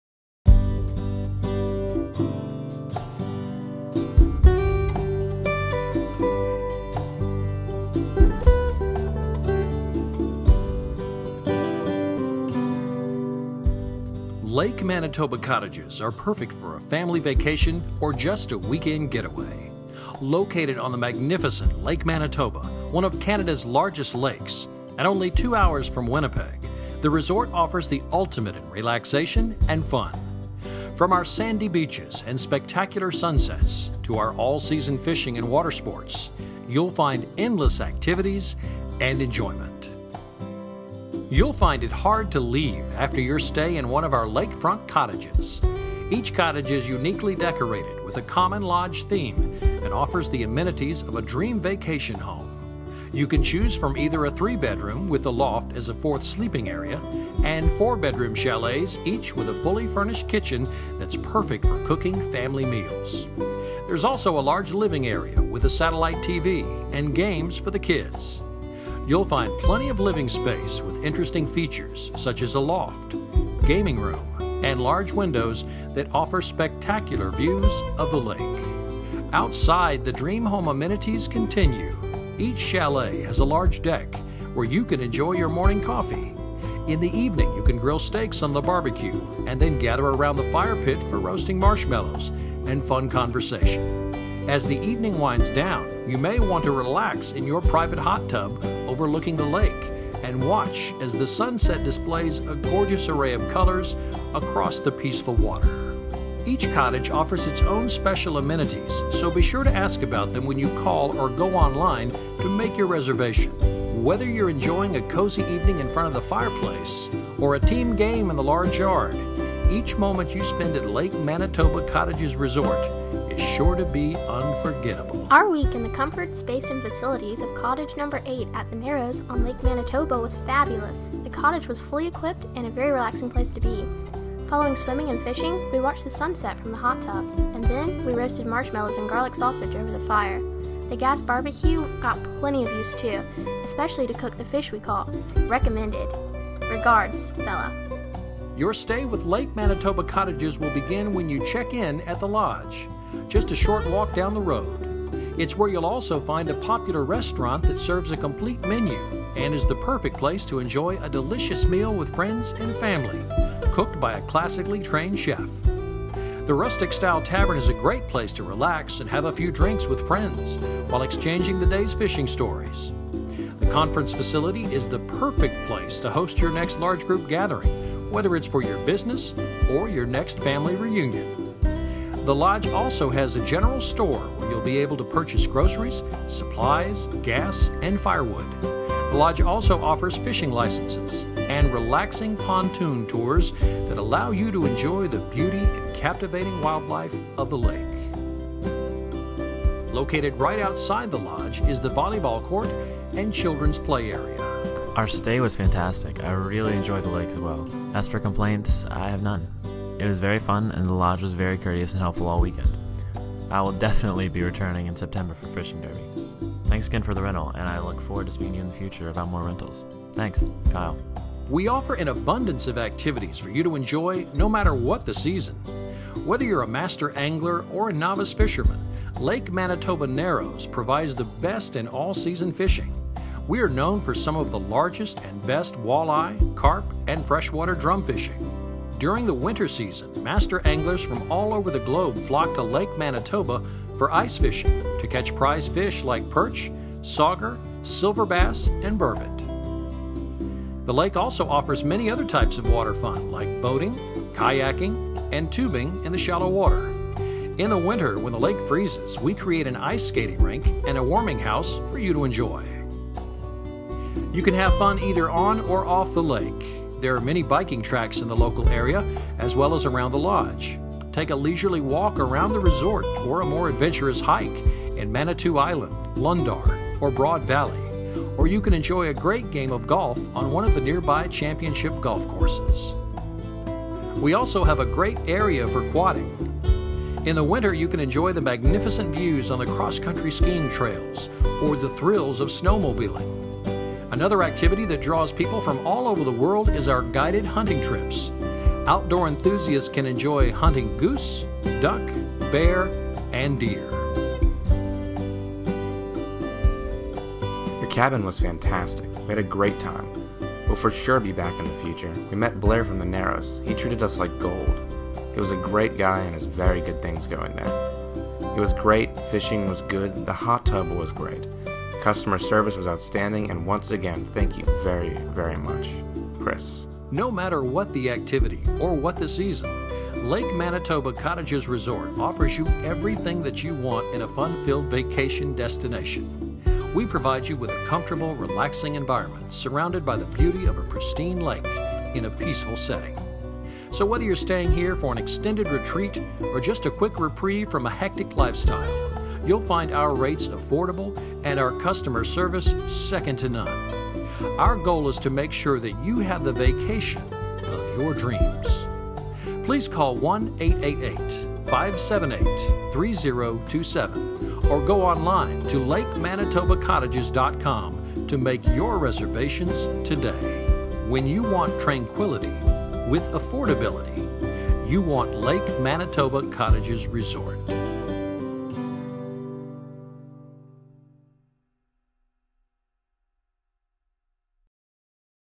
Lake Manitoba Cottages and Resort
Promotional Video for Lake Manitoba Cottages and Resort in Canada.
Tammy Hudson- Writer/Producer